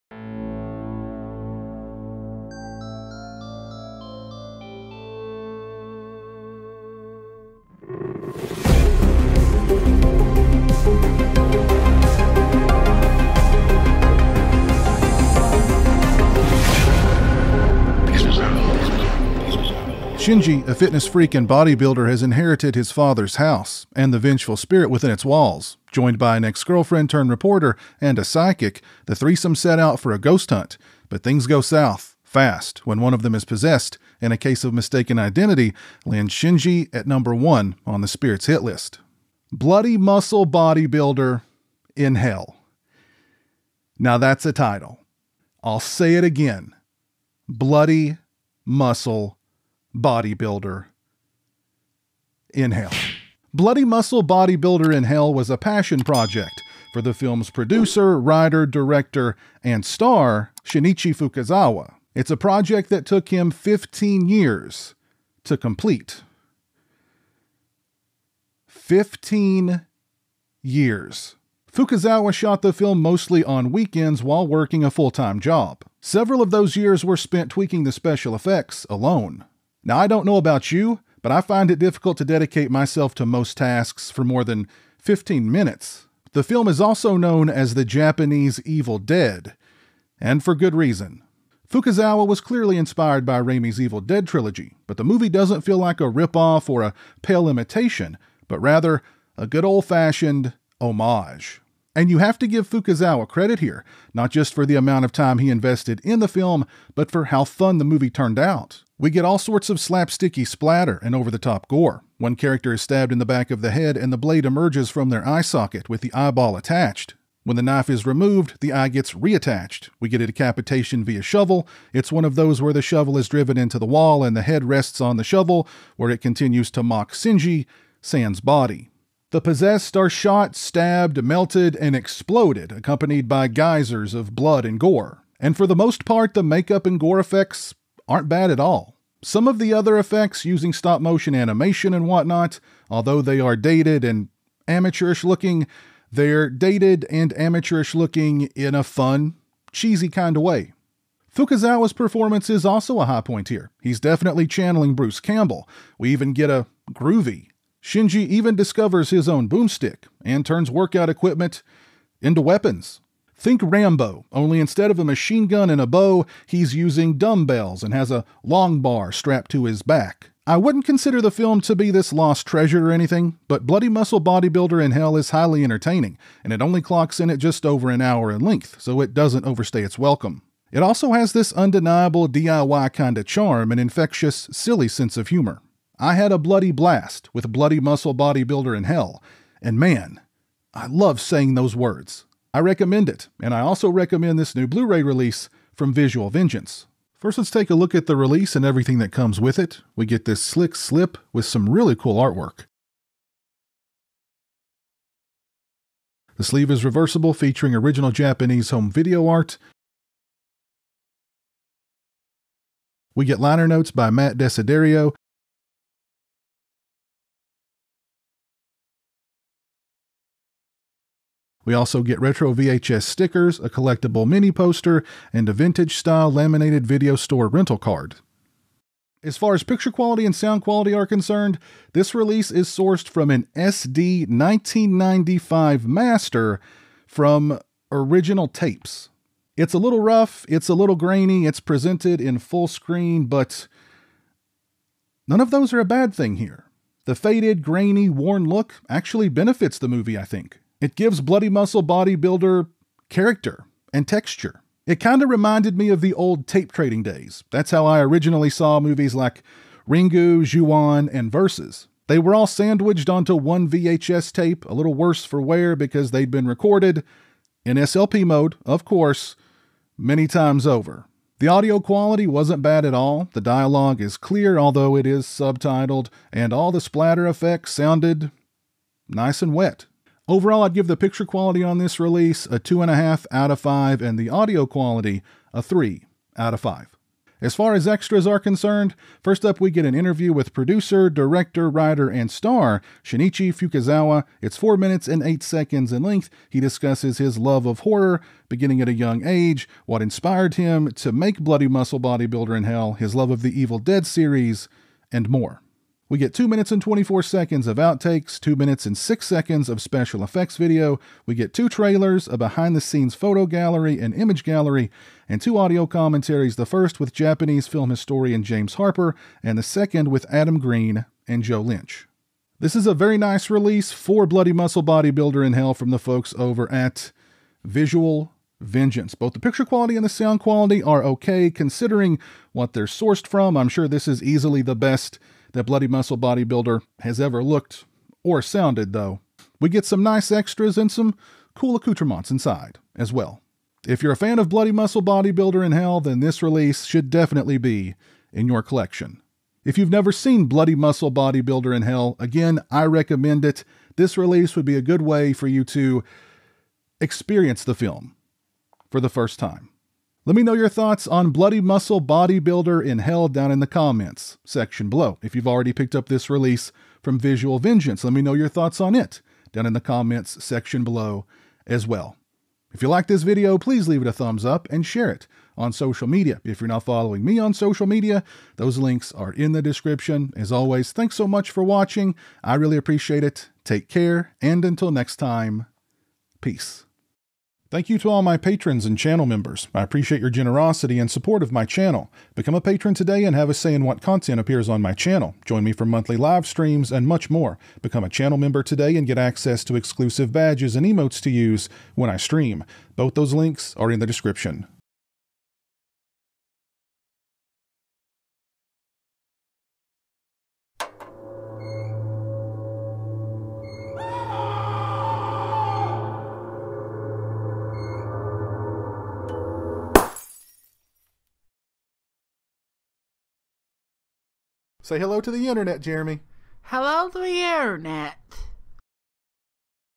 BLOODY MUSCLE BODYBUILDER IN HELL (2012) - Movie/Blu-ray Review (Visual Vengeance)
Pizowell reviews the movie and the new Visual Vengeance Blu-ray release for BLOODY MUSCLE BODYBUILDER IN HELL. Written, directed, produced and starring Shinichi Fukazawa.

► Amazon Wish List (by request)

● Audio interface
● Editing software

If you liked what you saw, show your support by liking and sharing the video. 👍